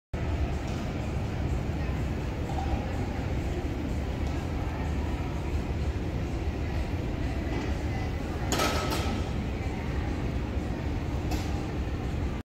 Carry - Unilateral Kettlebell Front Rack
COACHING CUES:
-Clean a kettlebell into your front rack position on one side.
-Elbows positioned downwards.
-Walk forward maintaining a rigid trunk.